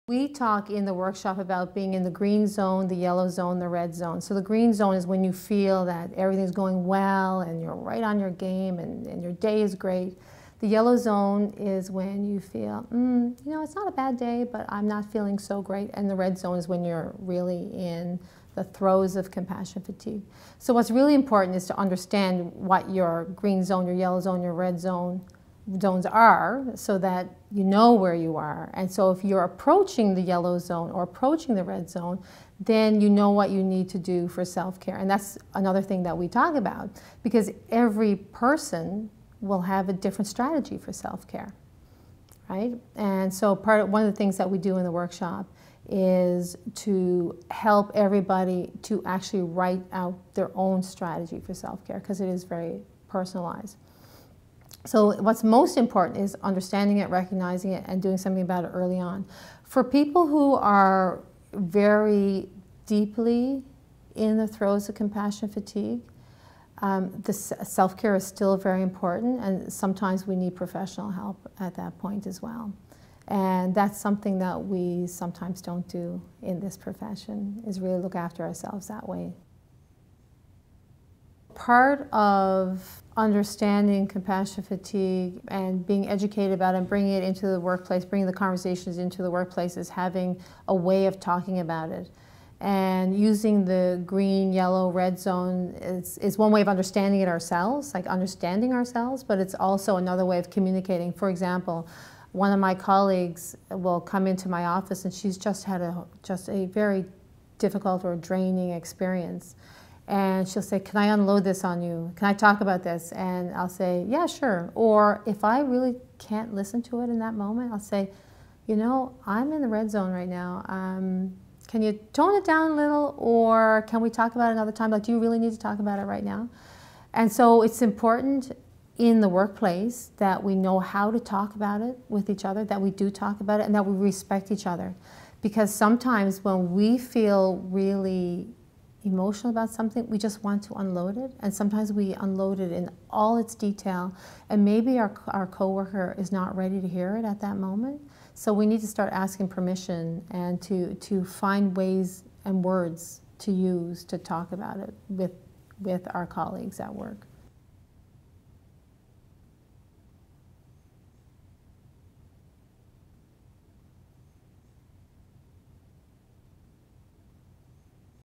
Addressing compassion fatigue
Dr. Tara Tucker, MD, Med, FRCPC, Director of the Faculty Wellness Program, Faculty of Medicine at the University of Ottawa and Assistant Professor in the Division of Palliative Care, Department of Medicine at the University of Ottawa, explores compassion fatigue with Virtual Hospice. She discusses the importance of self awareness, education, and support methods.